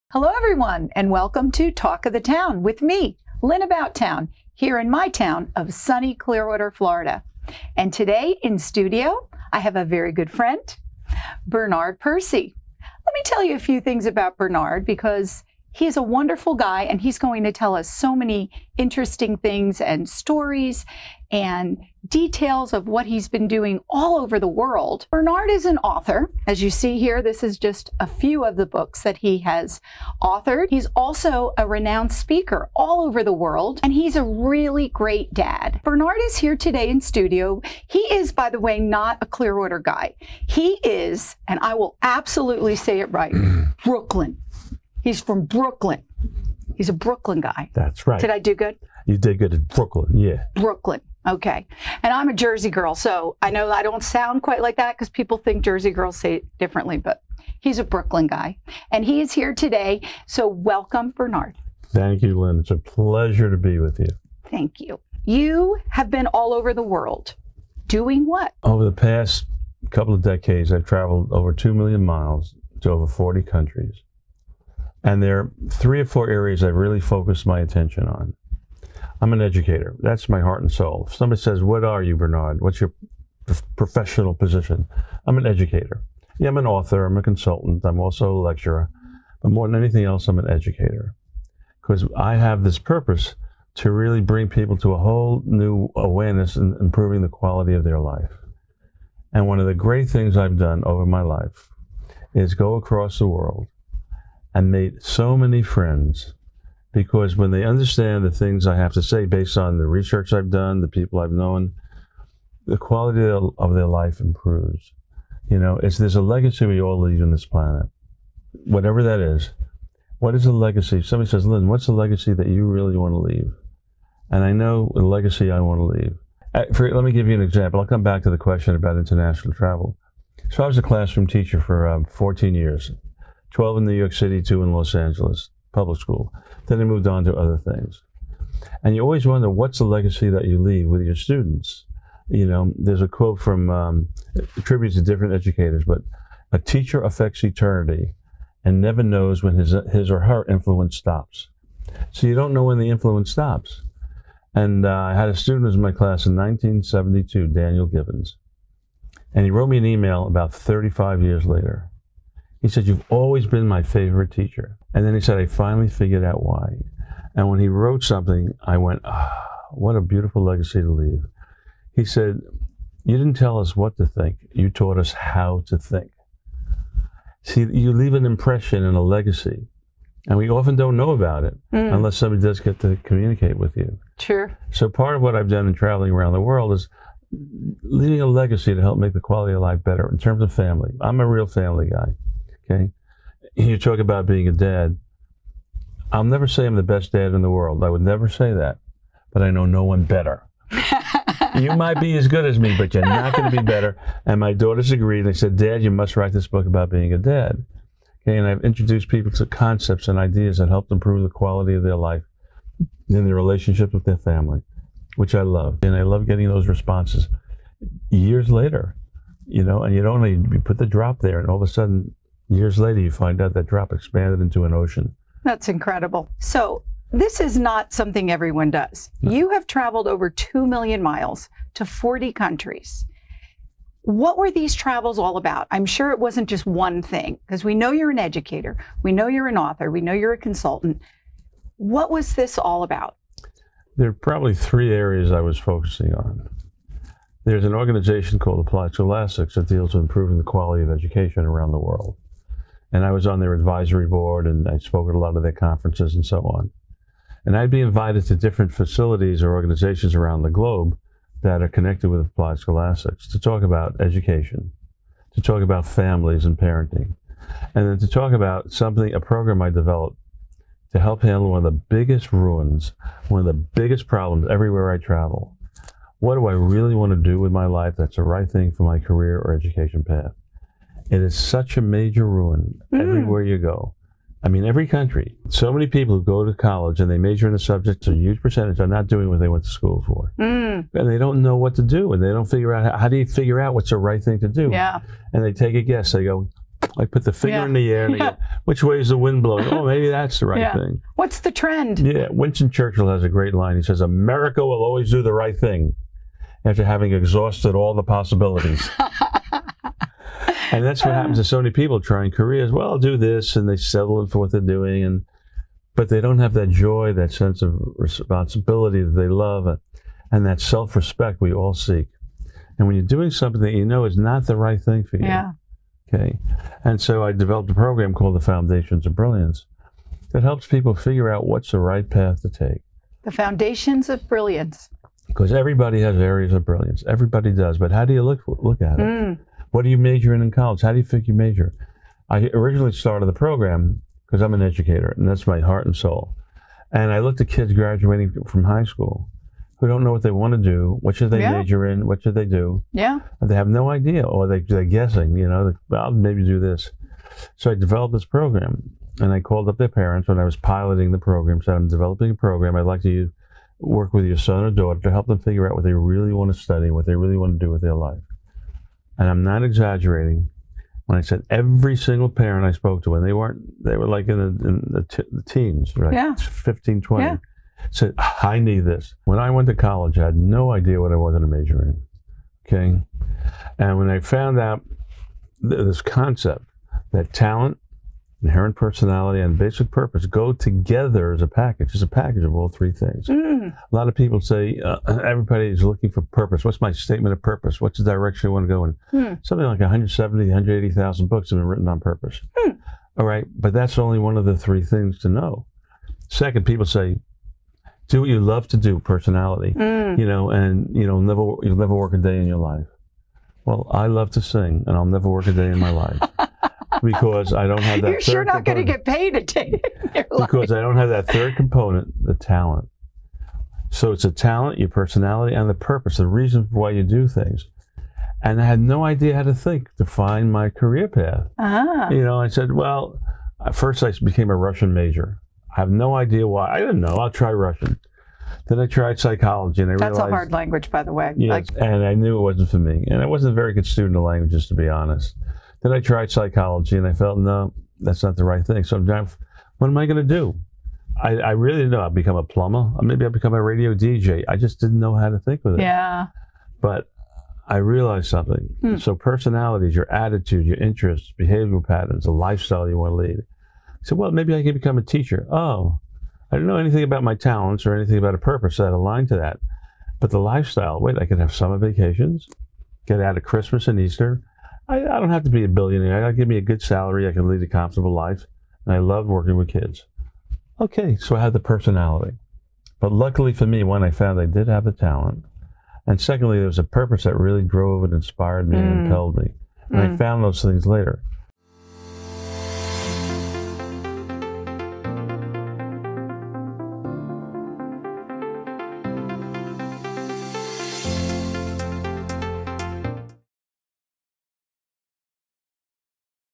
How Bernard Percy Traveled the Globe Helping Improve the Life of Others
This video was filmed before March 2020.

Bernard Percy has been all over the world, helping hundreds of people. He joins us to give us some tips and advice to apply to our lives!

Looking to purchase one of his books?